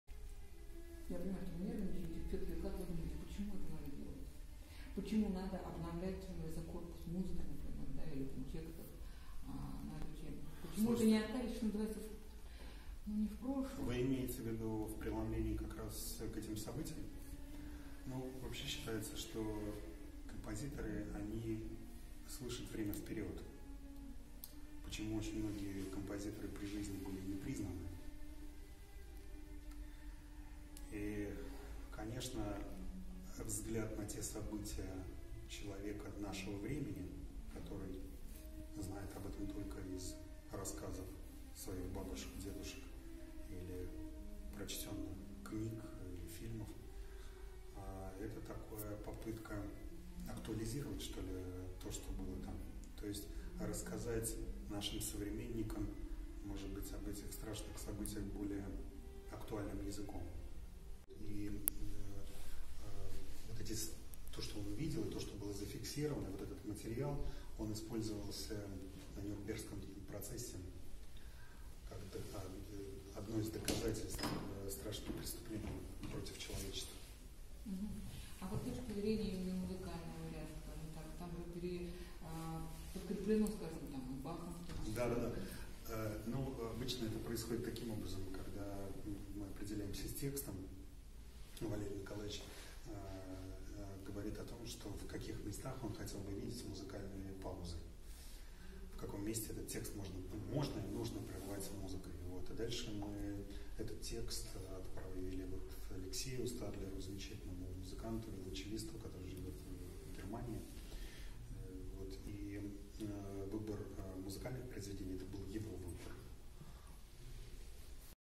Вячеслав Зильберборд о проекте "Желтые звезды"
Вячеслав Зильберборд рассказал о проекте "Желтые звезды"